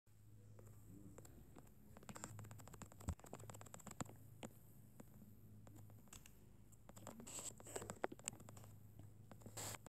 Trying stop motion Read DESCRIPTION
I mess up the stop motion but if you wish to see it you may its fast though